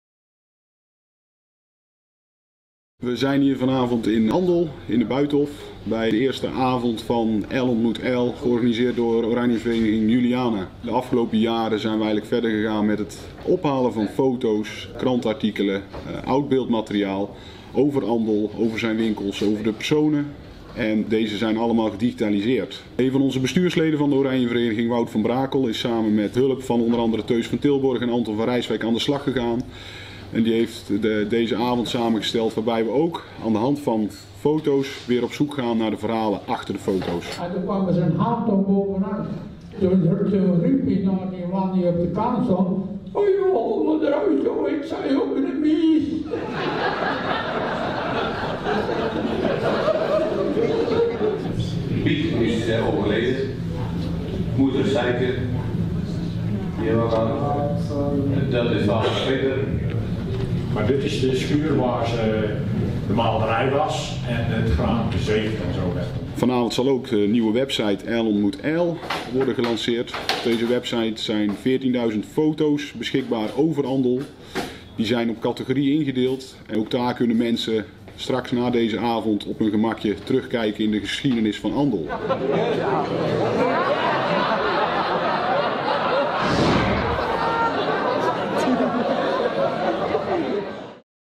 De historie van Andel in beeld tijdens Ael ontmoet Ael - Altena TV
ANDEL – Op vrijdag 18 januari was de officiële onthulling van de Ael ontmoet Ael website. Met ruim 14.000 foto’s die door de jaren heen zijn verzameld.

Tijdens een drukbezochte avond in ‘t Buitenhoff in Andel werden er diverse foto’s getoond en bijhorende verhalen verteld. Tijdens deze avond konden bezoekers eventueel aanvullende informatie geven om de historie nog beter in beeld te brengen. Op de website van Ael ontmoet Ael zijn vanaf nu de foto’s te bekijken die ingedeeld zijn diverse categorieën.

Wie thuis nog oud beeldmateriaal heeft en dit beschikbaar wil stellen, kan het materiaal beschikbaar stellen aan Oranjevereniging Juliana. Wanneer de foto’s zijn  gedigitaliseerd krijgt men het origineel weer terug.